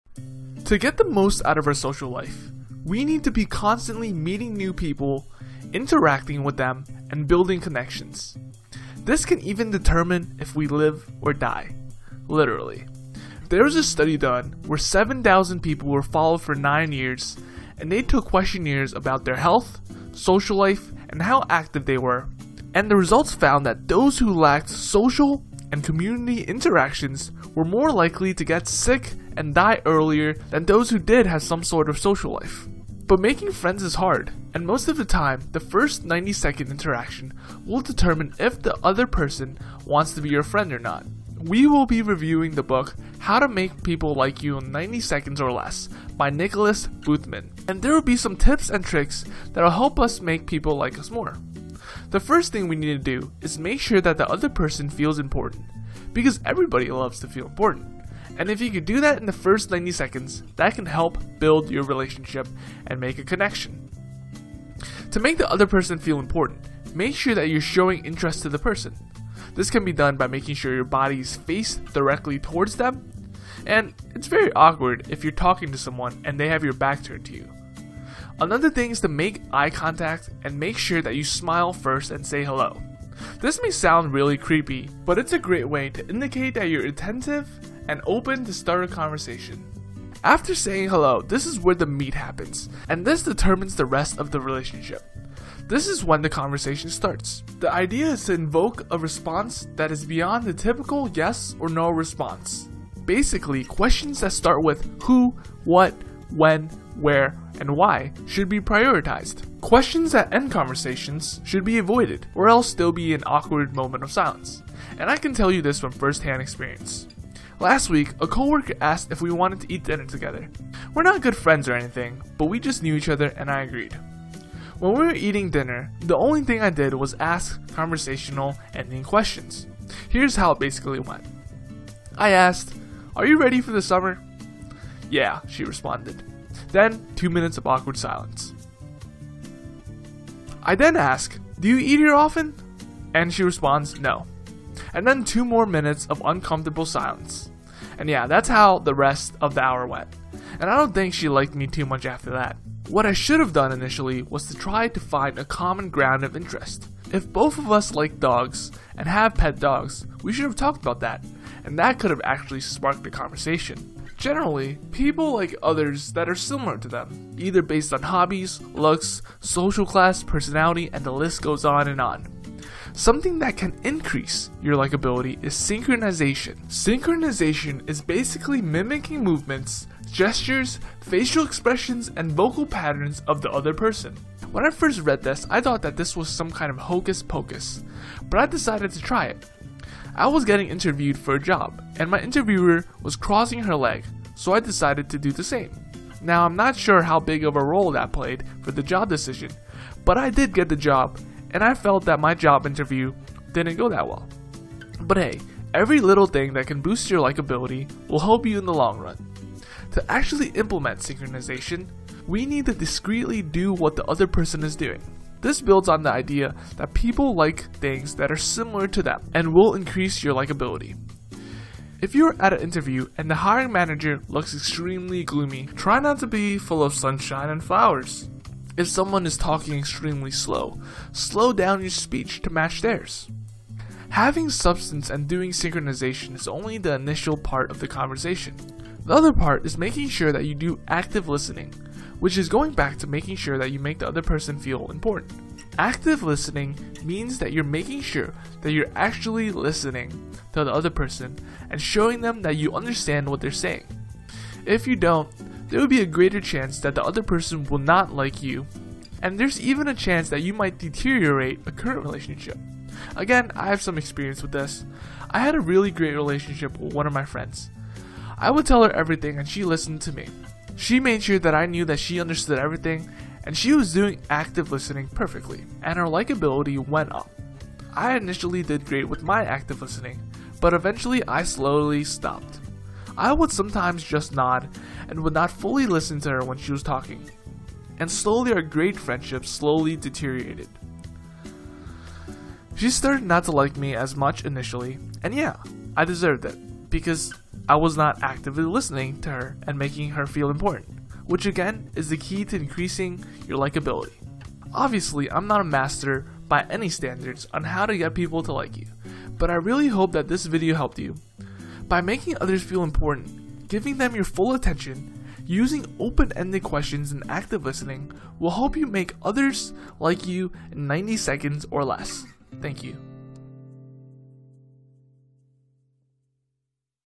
How to Make People Like You in 90 Seconds or Less
It is nerve racking when we are meeting new people and wondering if they like us or not. We are afraid to act normally, thinking that we will do something stupid, or we just do not know how to start a conversation and how to introduce ourselves without being a creep.

Hopefully, this video will provide some insight. This is the review of the book How to Make People Like You in 90 Seconds or Less by Nicholas Boothman

To get the most out of our social life, we need to be constantly meeting new people, interacting with them and building connections. This can even determine if we live or die. Literally.

There was a study done where 7000 people were followed for 9 years and they took questionnaires about their health, social life, how active they were and the results found that those who lacked social and community relationships were more likely to get sick and die earlier than those who do have some sort of social life.